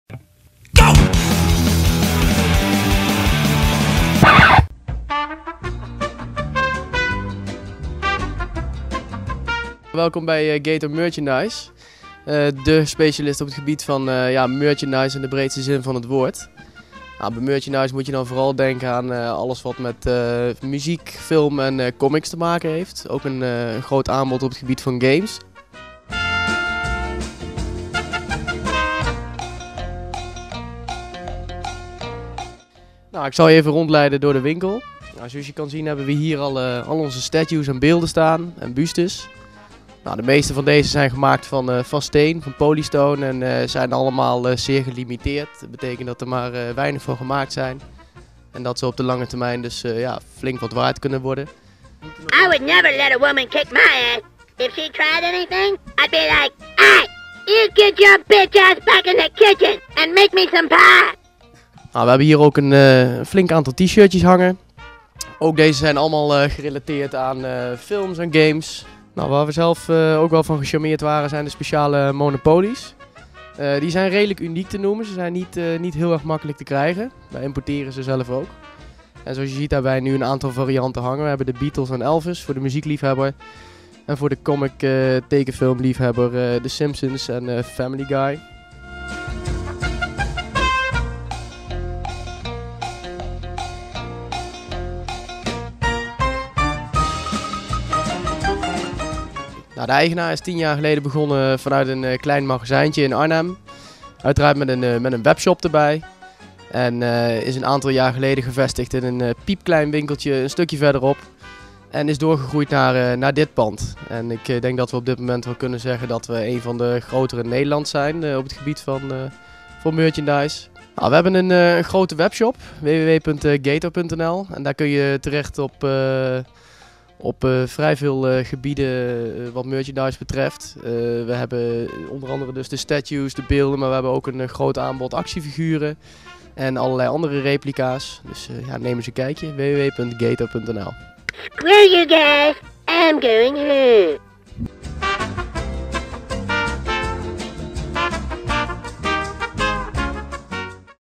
Winkelen met Jimmy: Gator film & game merchandise‎
Deze week winkelen we met Jimmy in Nijmegen. Mocht je nog lege plekken aan je muur hebben, of een verjaardagscadeau nodig hebben voor die gamende vriend van je, of misschien ben je zelf wel into de film en game merchandise. In ieder geval is Gator de winkel voor jou.

Gator is al 10 jaar een autoriteit op het gebied van film en game merchandise. Posters, t-shirts, collectibles, replicas, gadgets; ze hebben het allemaal. Wat dacht je van een krijsende Cartman-pop of een Family Guy editie van Monopoly? Check ook zeker Gator.nl om te kijken of er ook wat voor jou bij zit.